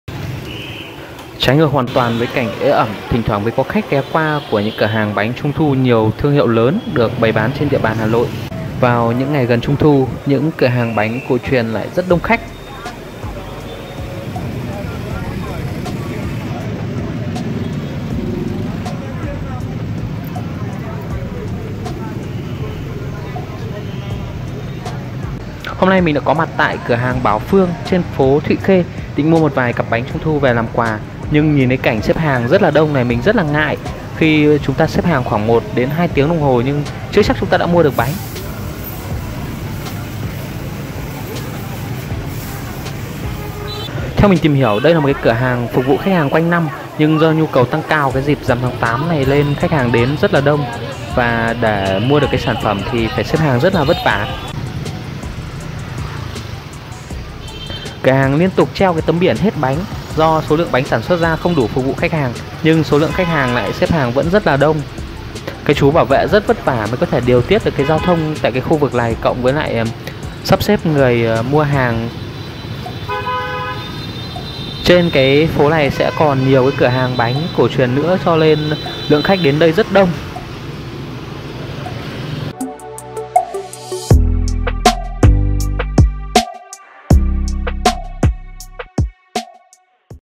Rồng rắn xếp hàng mua Bánh Trung Thu Bảo Phương - Thụy Khuê
Trái ngược hoàn toàn với cảnh ế ẩm, thỉnh thoảng mới có khách ghé qua của nhưng cửa hàng Bánh trung thu của nhiều thuong hiệu lớn, được bày bán trên địa bàn Hà Nội. Vào những ngày gần trung thu, những cửa hàng bán bánh cổ truyền lại rất đông khách. Hôm nay, mình có mặt tại cửa hàng Bảo Phương trên phố thụy khuê định mua một vài cặp bánh trung thu về làm quà, nhưng nhìn cảnh xếp hàng khá là đông, mình hơi ngại. Được biết đây là cửa hàng phục vụ khách hàng quanh năm, nhưng do nhu cầu tăng cao vào dịp rằm tháng 8 nên khách hàng rất vất vả mới có thể mua được sản phẩm. Cửa hàng liên tục treo tấm biển “Hết bánh”, do số lượng bánh sản xuất không đủ phục vụ khách hàng. Nhưng lượng khách hàng xếp hàng vẫn rất đông
Chúng ta cùng kết bạn: